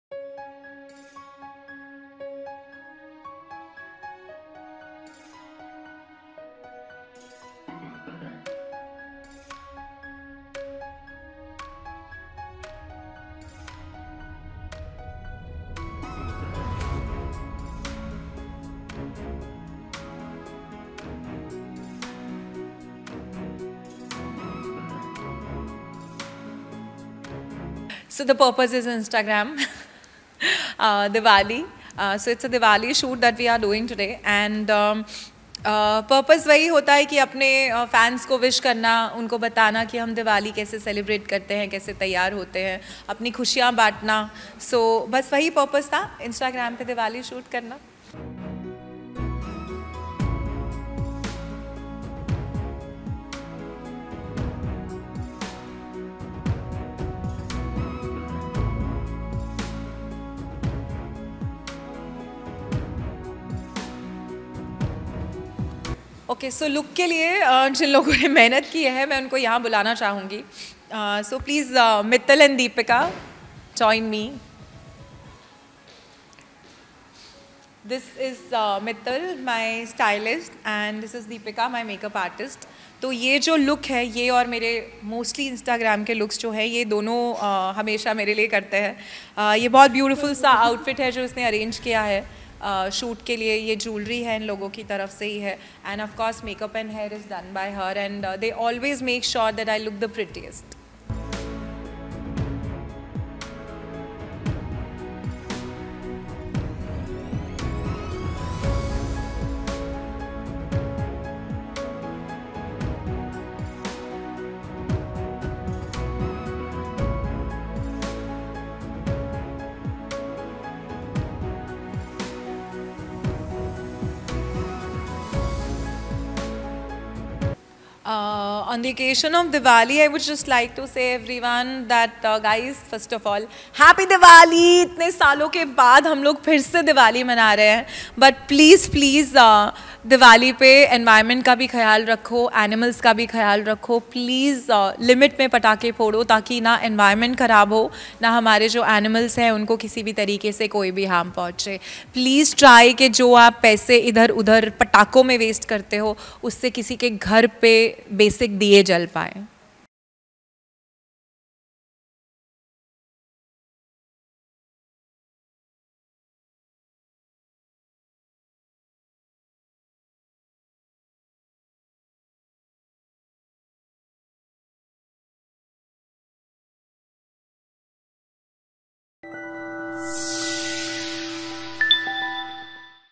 Taarak Mehta’s Priya Ahuja is festive ready in a stunning red outfit
Taarak Mehta Ka Ooltah Chashmah fame Priya Ahuja, who rose to fame for her role of Rita reporter, got all festive ahead of Diwali. The actress donned a gorgeous red ethnic outfit and looked amazing. She shared with us the behind the scenes from her new photoshoot. The actress also extended Diwali wishes to everyone and urged fans to consider gifting things to the poor people this time.